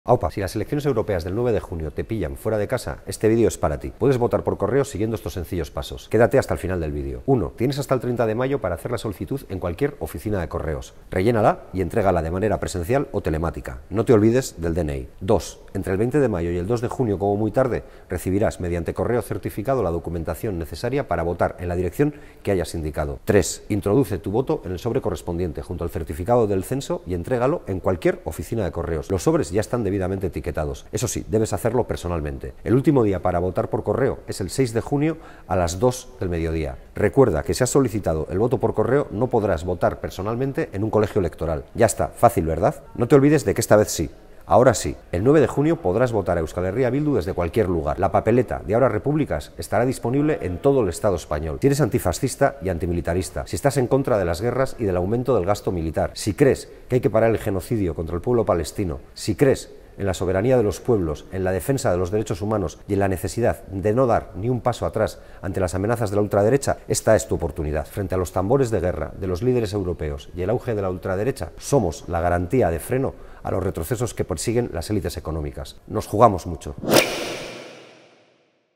AhoraRepúblicas | OSKAR MATUTE explica cómo votar por correo en las elecciones europeas
Si eres antifascista, antimilitarista y crees en la soberanía de los pueblos, esta es tu oportunidad de votar a EH Bildu.

En las elecciones europeas del 9 de junio alzaremos la voz ante los tambores de guerra de las élites europeas y el auge del ultraderecha.